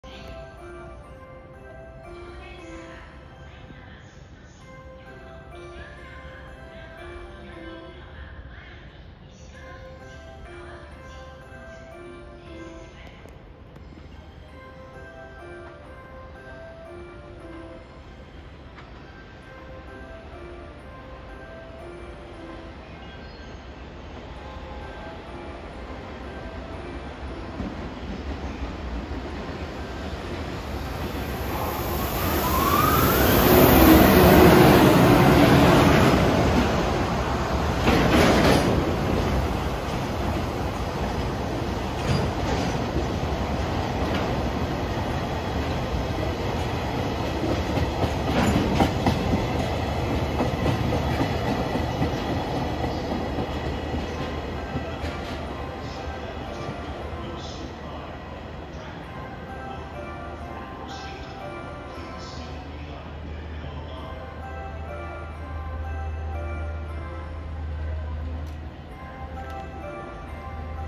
JR貨物EH500形33号機＋タキ11B（両）が大宮駅4番線を通過するシーン（臨8072レ・仙貨A115・石油返空）2022.11.29
今日もコキ無しか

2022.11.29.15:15